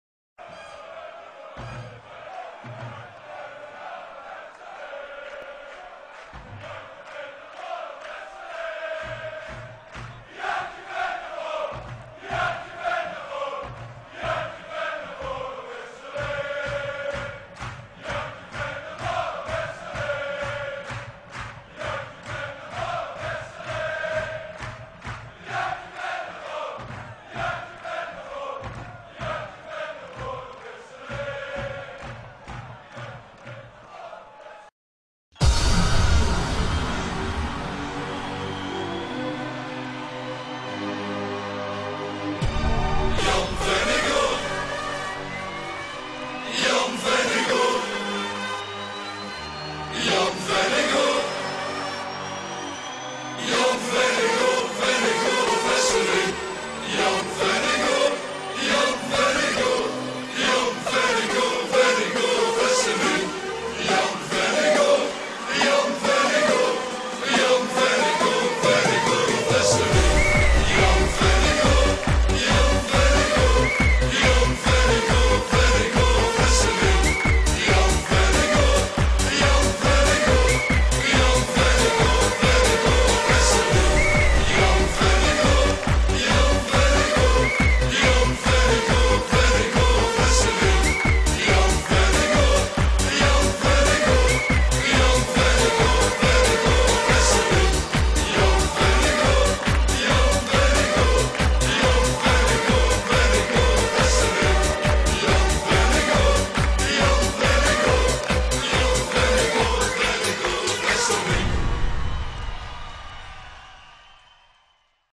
Jan Vennegoor Of Hesselink - SK Rapid Wien
Der erste Tag von Jan Vennegoor of Hesselink beim SK Rapid Wien (Dienstag, 31. August 2010)! Die Verpflichtung wurde am Tag davor bekannt gegeben. In diesem Video findet ihr alle Bilder von Tag 1 mit VoH in Hütteldorf. Vom Training über Autogrammstunde bis Pressekonferrenz und Präsentation des Holländers. Viel Spaß mit unserer neuen Nummer 10!